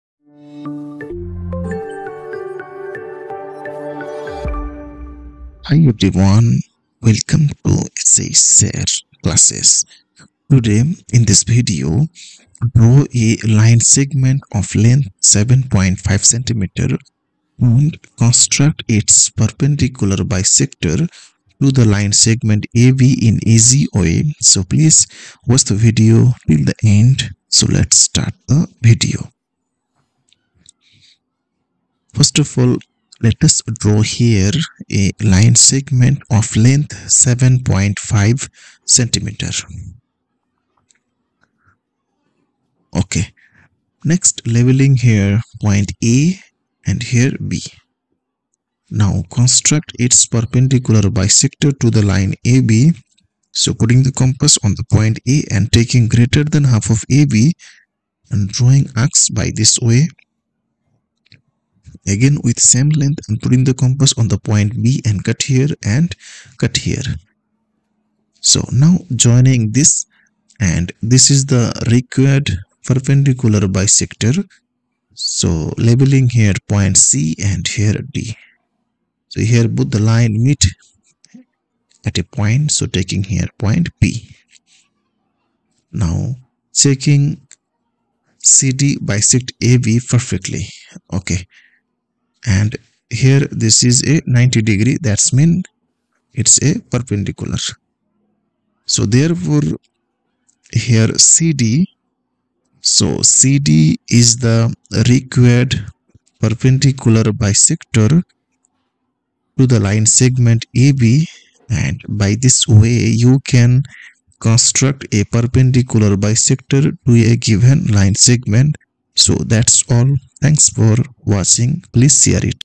Draw a line segment of length 7.5 cm and construct its perpendicular bisector.
In this video how to draw a line segment of length 7.5 cm and construct its perpendicular bisector. Geometrical Construction. @SHSIRCLASSES .